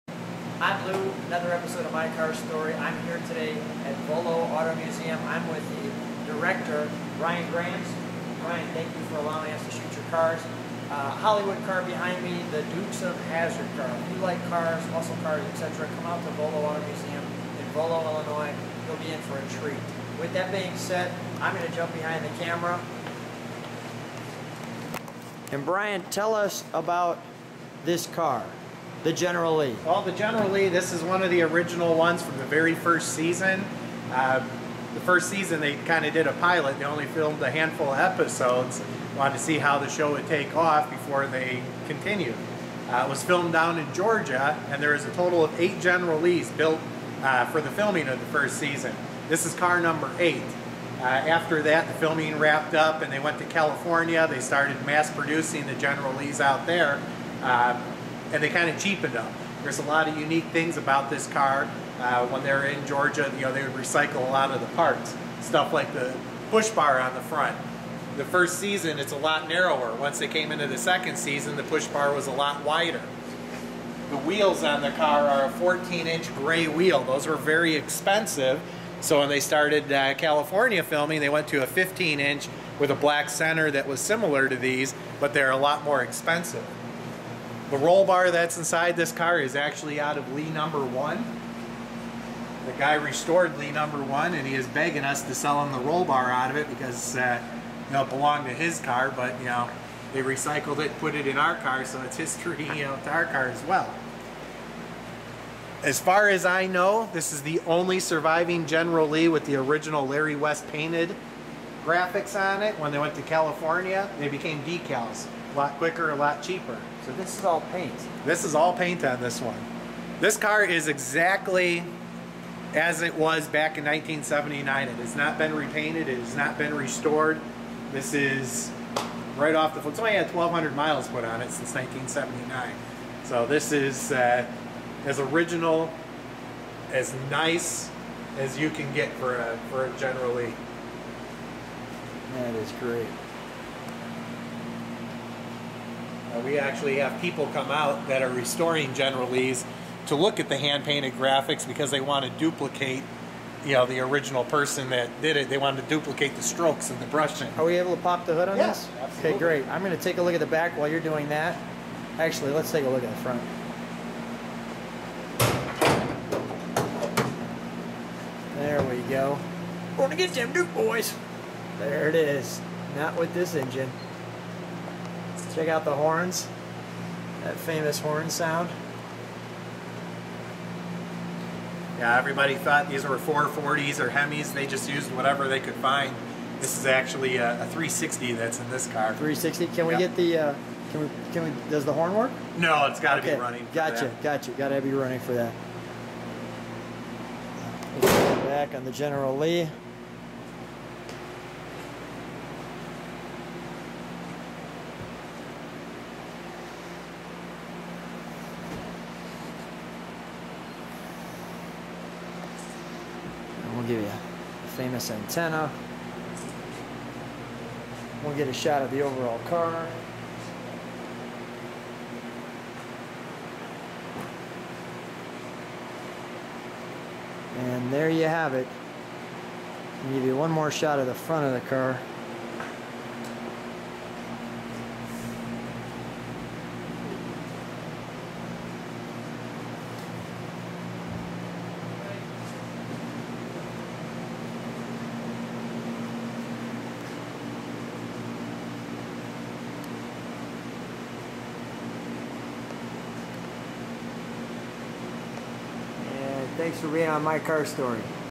1969 Dodge Charger General Lee from the TV show Dukes of Hazzard on My Car Story with Lou Costabile
On "My Car Story" we're in Volo IL on 2-10-14.

We're looking at a My Car Story with Lou Costabile 1969 Dodge Charger General Lee from the TV show The Dukes of Hazzard from January 26, 1979 to February 8, 1985 driven by Luke Duke, played by Tom Wopat, and Bo Duke, played by John Schneider.

Today I'm speaking with Brian Grams, Director of the Volo Auto Museum.

The car's located at Volo Auto Museum, 27582 Volo Village Rd., Volo IL 60073.

If you love cars (Hollywood, Classic, Muscle), and Military equipment, memorabilia, and mementos then you must stop by this location.